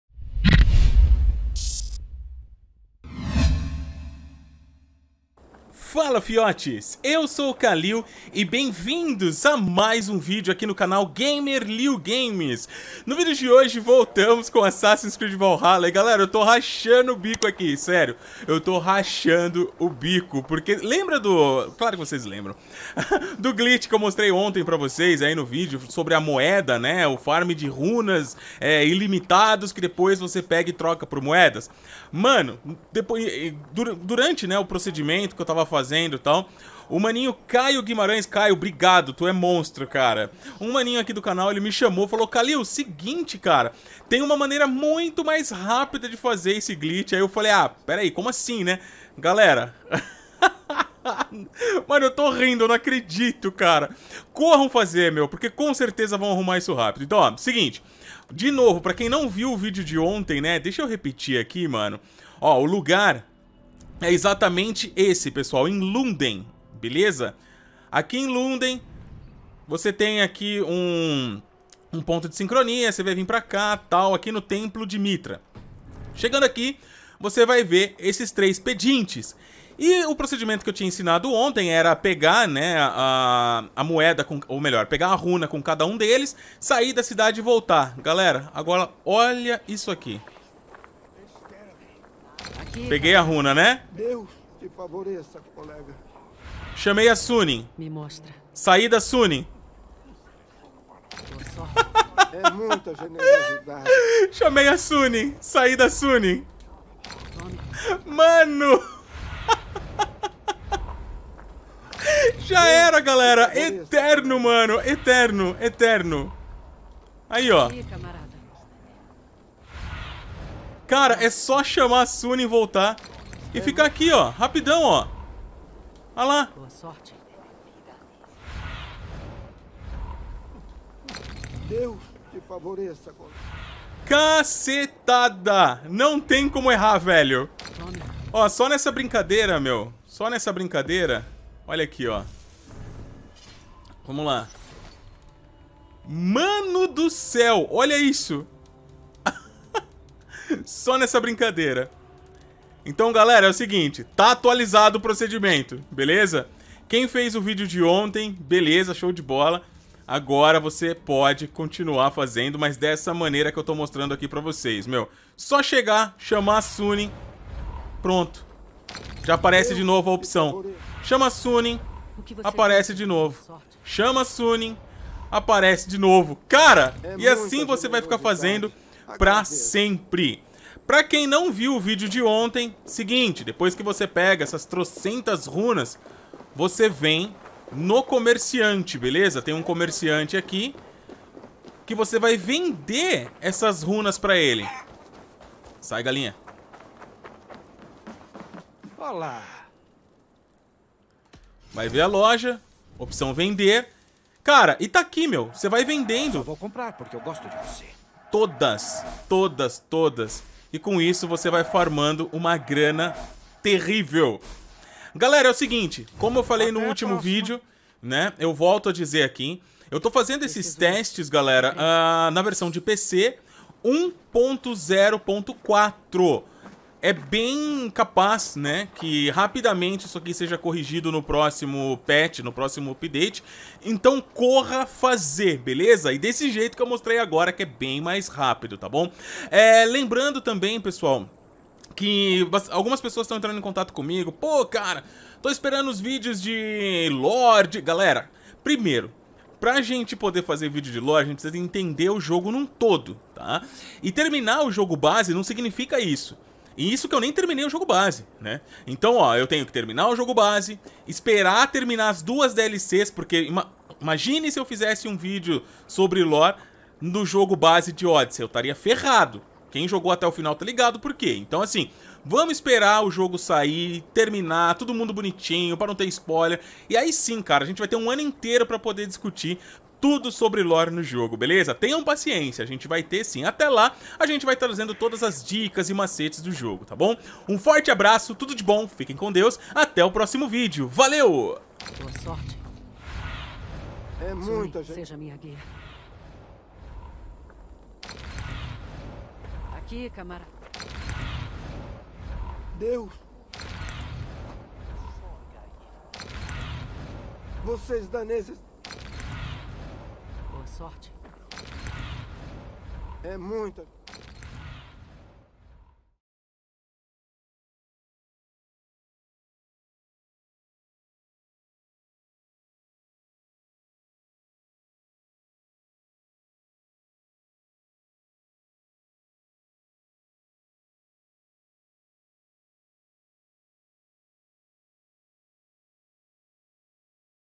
MÉTODO MUITO MAIS RÁPIDO PARA RUNAS E MOEDAS ILIMITADAS!!! FAÇA RÁPIDO!!! (v.1.0.4)
Fala Fiotes!
Como estão?
Segue um vídeo rápido que fiz para mostrar como farmar de maneira INFINITA uma runa específica e moedas no jogo.
Sim, se trata de um macete que provavelmente em próximas atualizações será removido.
Precisa de grana? Bom, é agora!
Não se esqueçam de comentar no vídeo se funcionou com você, qual a plataforma usada e data também para que mais pessoas estejam cientes desses detalhes ao olharem os comentários futuramente.

Kallil Augusto